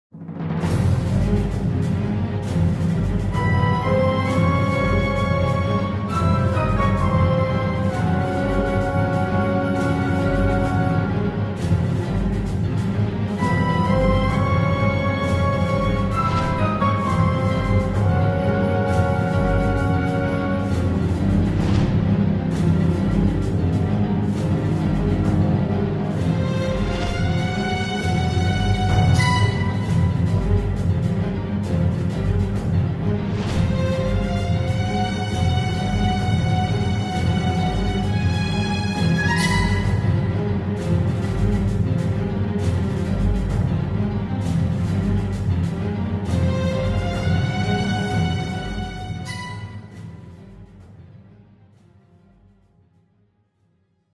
2017 Nissan 370Z Interior
Take An In Depth Tour Of The Interior Of Our Pre-Owned 2017 Nissan 370Z / ST#955335